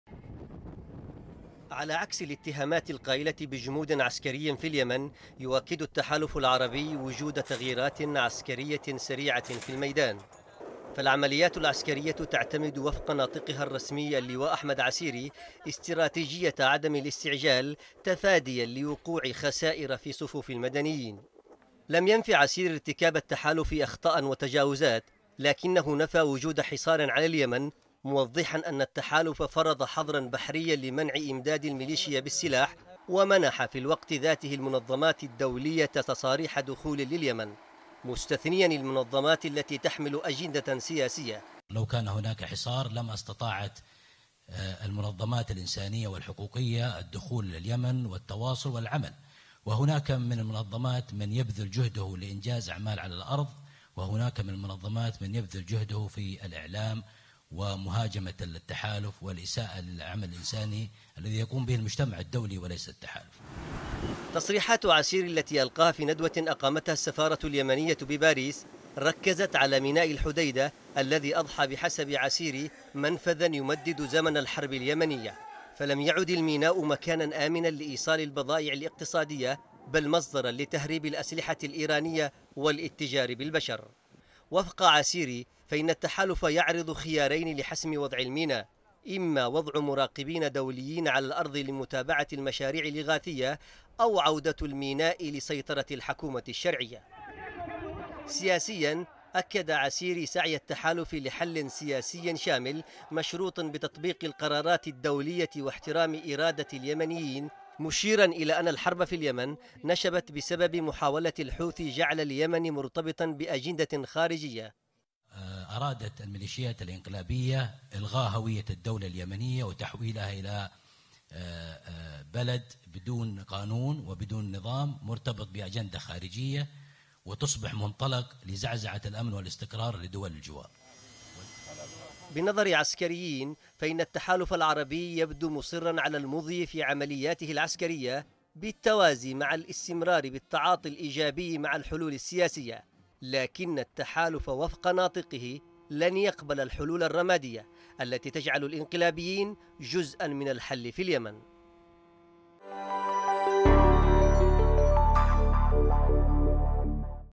التحالف العربي يؤكد رفضه الحلول السياسية الرمادية | تقرير: محمد اللطيفي
المتحدث باسم التحالف العربي اللواء أحمد عسيري يدعو إلى نشر مراقبين دوليين في ميناء الحديدة أو إعادة سيطرة الحكومة الشرعية عليه، متهماً المليشيا بتهريب السلاح عبر الميناء.
وأكد عسيري في ندوة عقدت في باريس أن سياسة حصار المليشيا أدت إلى نتائج فعالة، مضيفا أن الاستعجال في تنفيذ الخطط العسكرية قد يؤدي إلى خسائر في صفوف المدنيين.